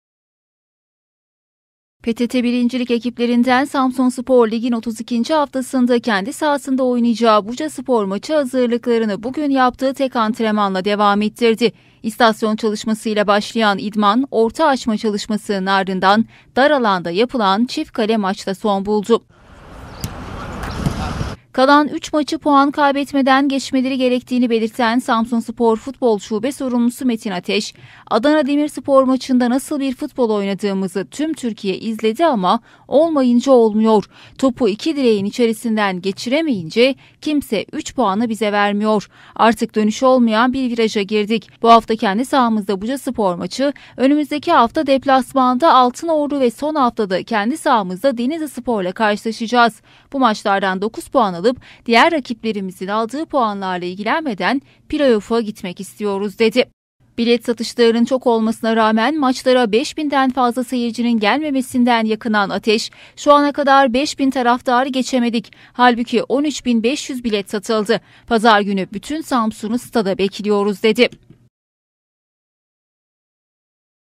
SAMSUNSPOR ZOR VİRAJDA
Samsunspor Futbol Şube Sorumlusu Metin Ateş, kalan 3 maçtan 9 puan toplayıp play-off’a kalmayı hedeflediklerini söyledi.